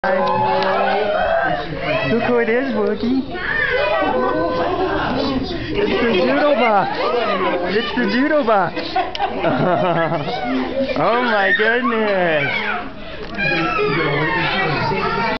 Owen's reaction to seeing the Doodlebops in person
at a meet and greet at the Sprint Center